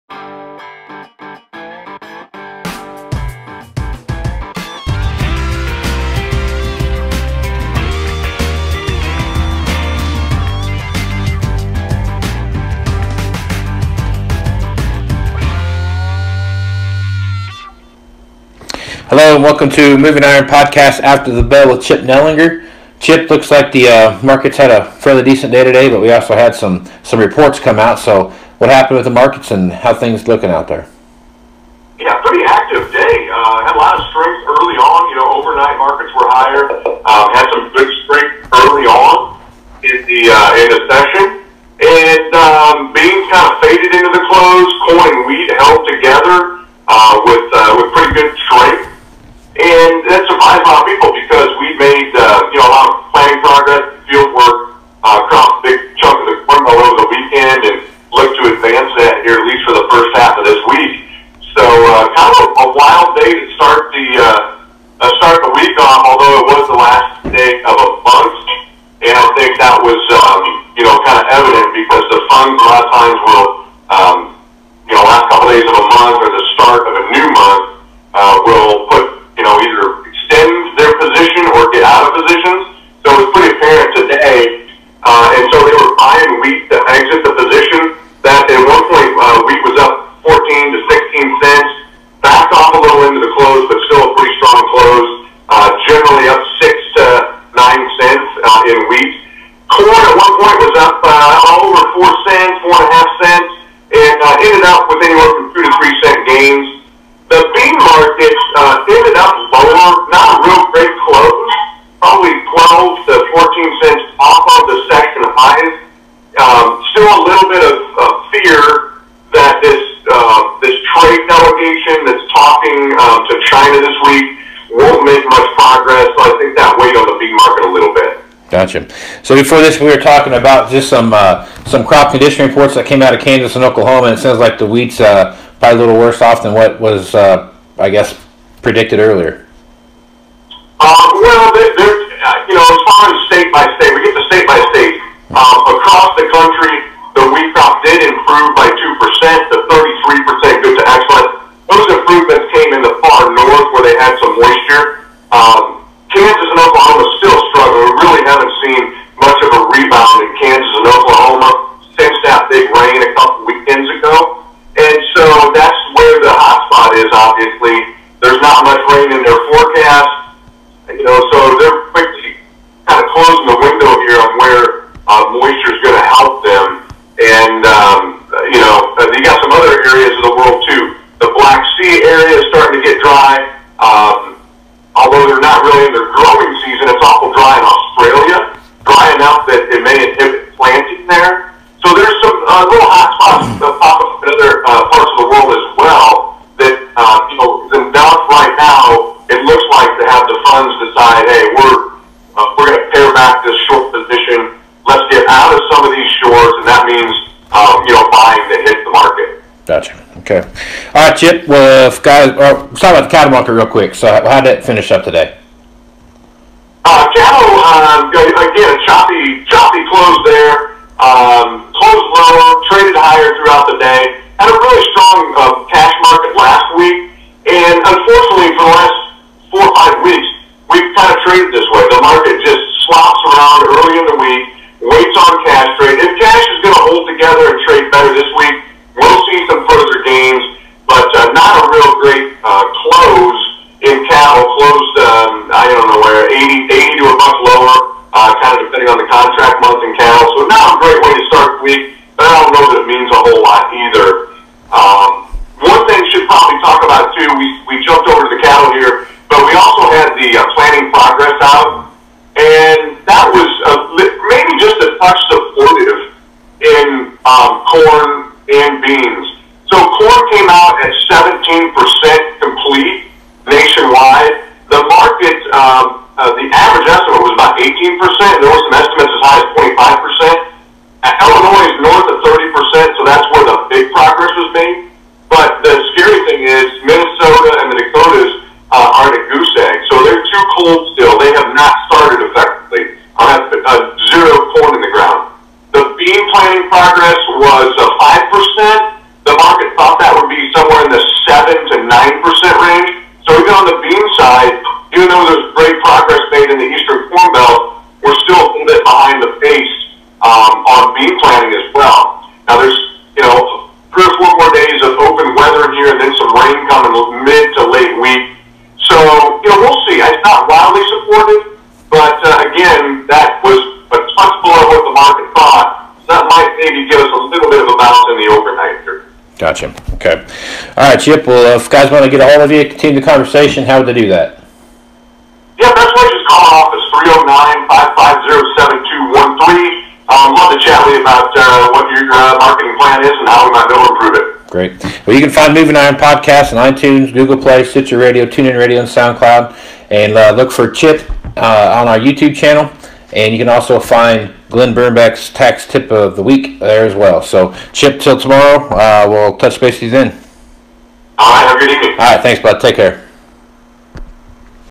Moving Iorn Podcast After the Bell with Chip Nellinger 4-30-18
Chip hits the highlights of the markets each day at 1:30 CST.